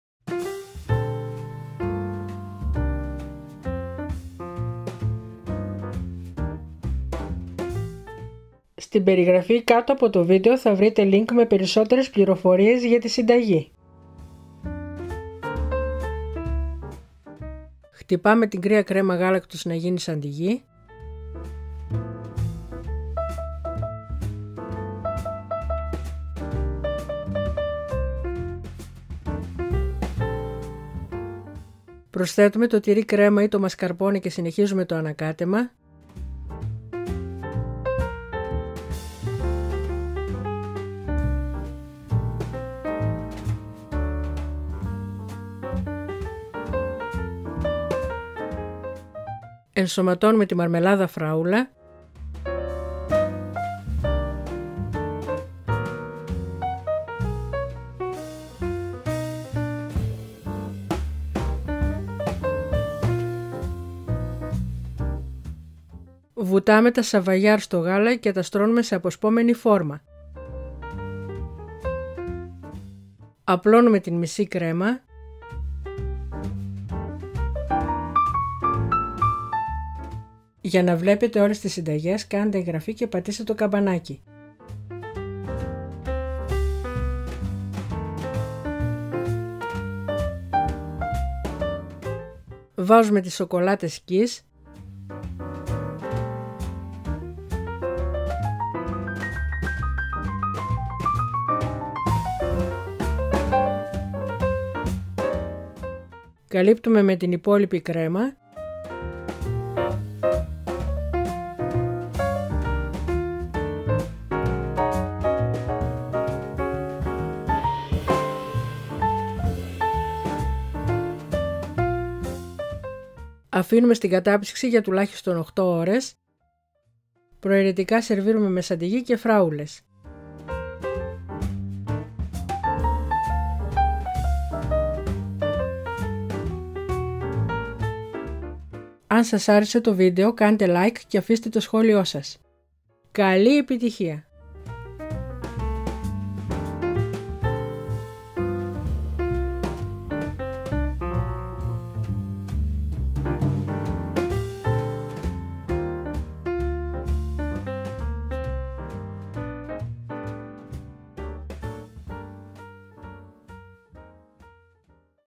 Semifreddo φράουλα και σοκολάτες kiss | Foodaholics
Semifreddo φράουλα και σοκολάτες kiss | Semifreddo φράουλα και σοκολάτες kiss | Foodaholics

 Συστατικά:

400 ml κρέμα γάλακτος
 200 γρ τυρί κρέμα
 200 γρ μαρμελάδα φράουλα
 6 kiss
 10 σαβαγιάρ
 Λίγο γάλα

  400 ml of cream
  200 g cream cheese
  200 grams of strawberry jam
  6 kiss chocolates
  10 Sabayar
  Some milk